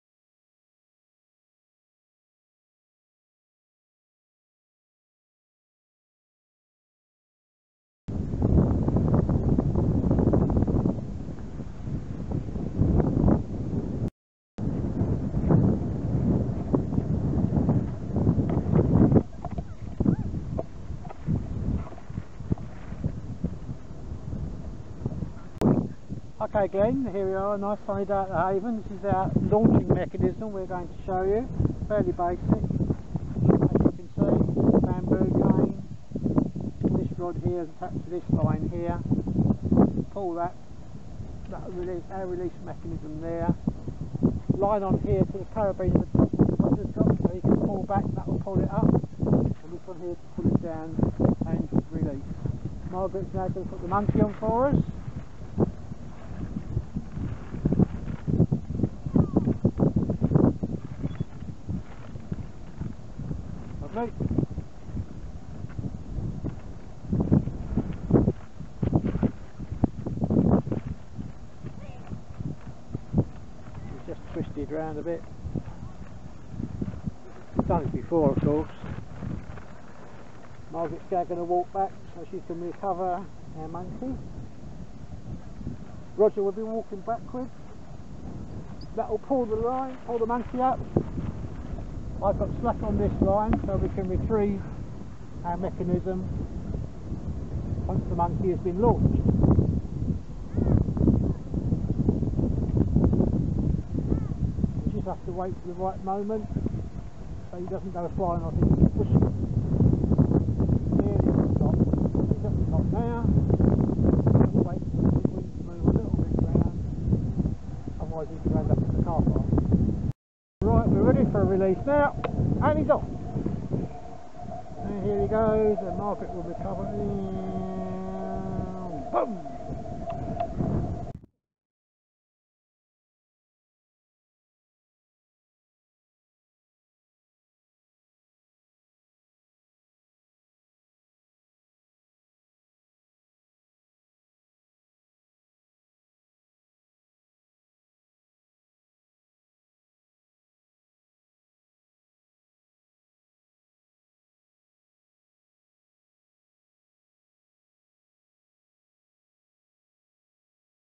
parachuting monkey rig
This is the rig we use for launching thrill seeking Parachuting Monkeys, other soft toys or doing sweet drops at festivals  - no Monkey was hurt during this film, in fact they are such adrenaline junkeys he insisted on having several goes